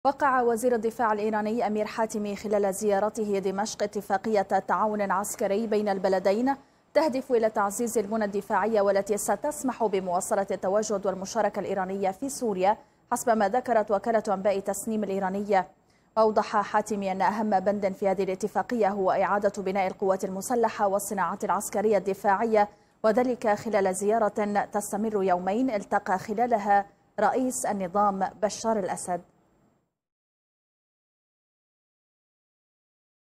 إيران ت اتفاقية لدعم صناعات النظام السوري الدفاعية
شاهد آيضا:
قيادي بعثي يفضح تاريخ آل الأسد!؟

وقع وزير الدفاع الإيراني أمير حاتمي خلال زيارته دمشق اتفاقية َ تعاون عسكري بين البلدين تهدف إلى تعزيز البنى الدفاعية، والتي ستسمح بمواصلة التواجد والمشاركة الإيرانية في سوريا، حسبما ذكرت وكالة أنباء "تسنيم" الإيرانية.

أوضح حاتمي أن أهمّ بند في هذه الإتّفاقية هو إعادة بناء القوات المسلّحة والصناعات العسكرية الدفاعية، وذلك خلال زيارة تستمر يومين التقى خلالها رئيس النظام بشار الأسد.

واعتبر حاتمي أن سوريا "تتخطى الأزمة وتلج إلى مرحلة هامة للغاية هي مرحلة إعادة البناء".

وذكرت وكالة الأنباء السورية الرسمية أن حاتمي وأيوب وقعا "اتفاقية تعاون مشترك بين جيشي البلدين" من دون التصريح بأي تفاصيل

وفي مقابلة مع قناة الميادين الفضائية جرى بثها مساء الإثنين، أوضح حاتمي من دمشق أن "أهمّ بند في هذه الإتّفاقية هو إعادة بناء القوات المسلّحة والصناعات العسكرية الدفاعية السورية لتتمكّن من العودة الى قدرتها الكاملة".